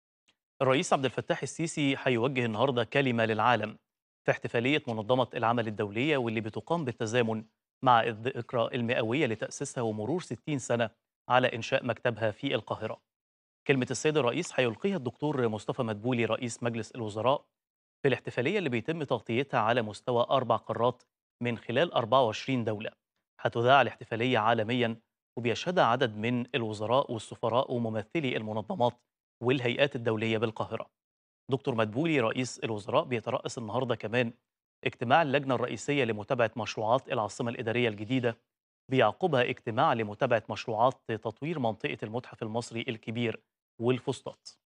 رئيس الوزراء يلقى كلمة الرئيس السيسي فى احتفال منظمة العمل الدولية بمصر
صباحك_مصري | رئيس الوزراء يلقى كلمة الرئيس السيسي فى احتفال منظمة العمل الدولية بمصر